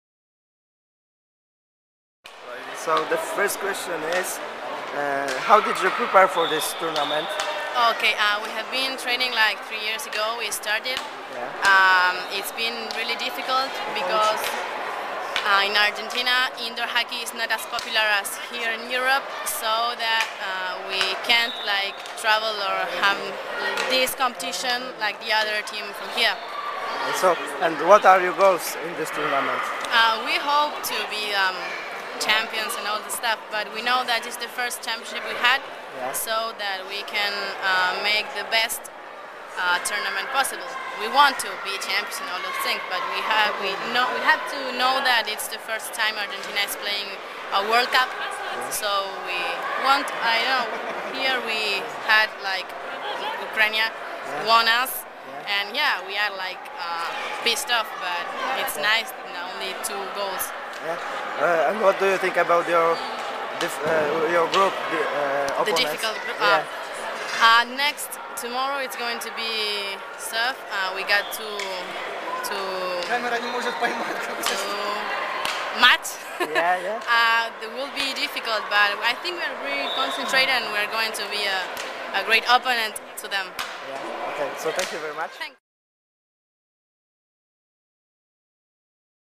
Mercedes PAZ (ARG) - 2011 FIH Indoor World Cup
Mercedes PAZ from Argentina speaks about the match against Ukraine, they lost. Twitter Twitter